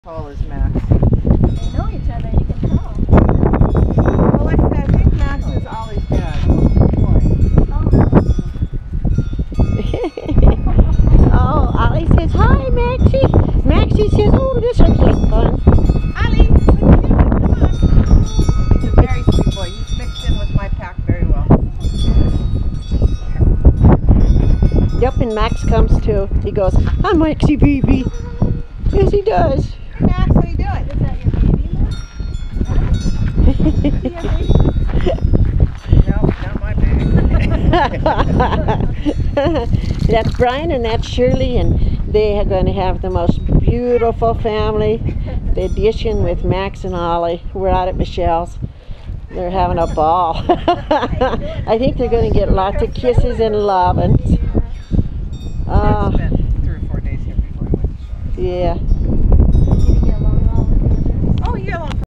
Max and Ollie go to their forever home